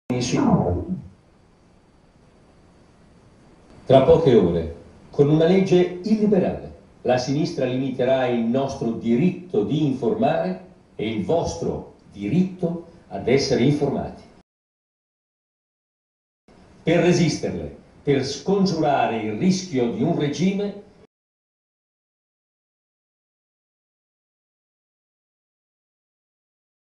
Spot di Articolo 21 contro il Ddl Alfano sulle intercettazioni
Da lunedì il Ddl Alfano sulle intercettazioni arriva alla discussione parlamentare. Uno spot per dire no al bavaglio. Diffondilo sulla rete in ogni spazio a tua disposizione